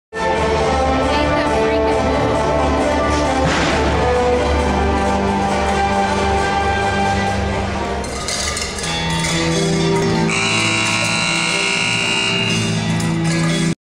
K Bar J Bucking Bulls 2011 - Rads 814 B
K Bar J Bucking Bulls.  Bull Rads 814.  Cowtown Colessium, Ft Worth, TX.